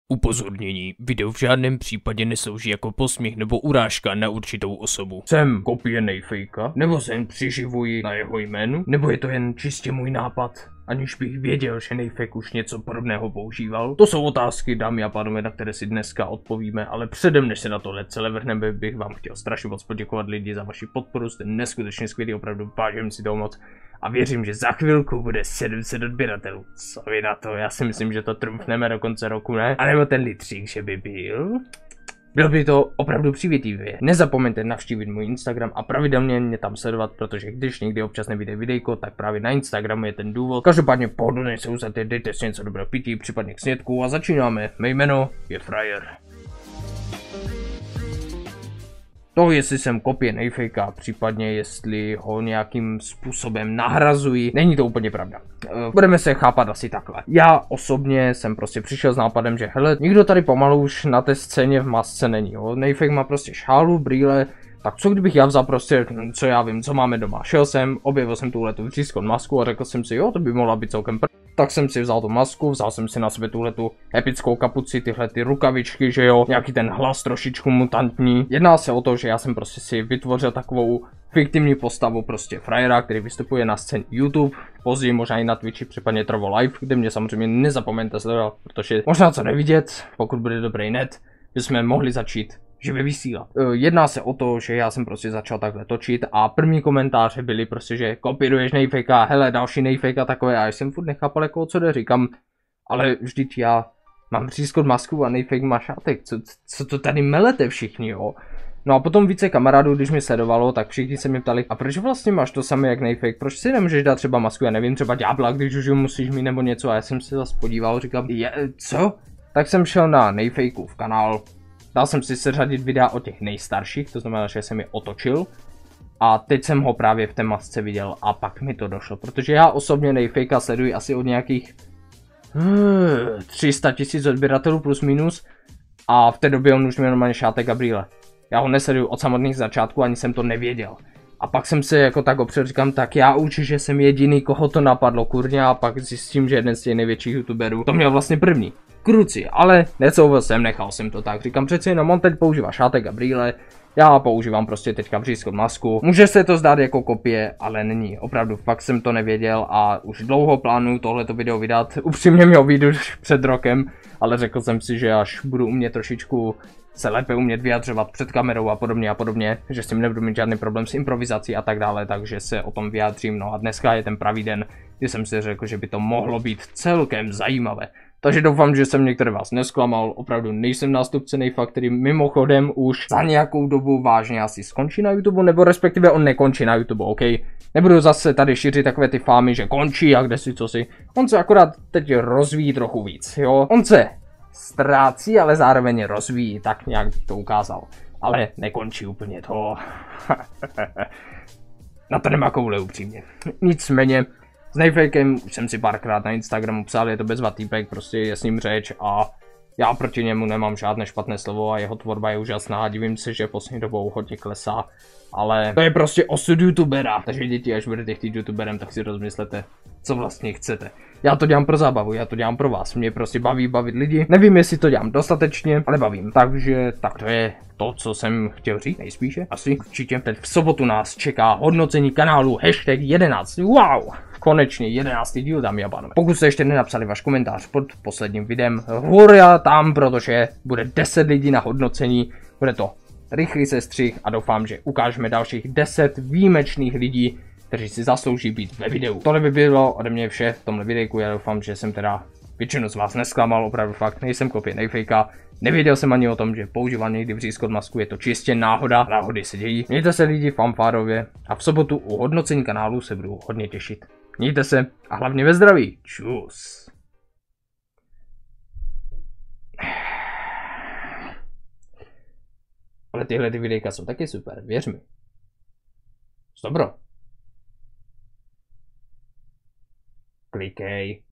Kopíruji NejFaka? | FRAJE®
⬇️PŘEČTI SI POPISEK⬇️
Ve videu vysvětluji to, na co se ptalo už hodně lidí.
Jsem kopie, příživník nebo dokonce podvodník?
To se dozvíte v dnešním videu:)
✅SOCIÁLNÍ SÍTĚ✅
📅ROZVRH📅
🆒PODPOŘTE TAKÉ🆒
©Fraje®2021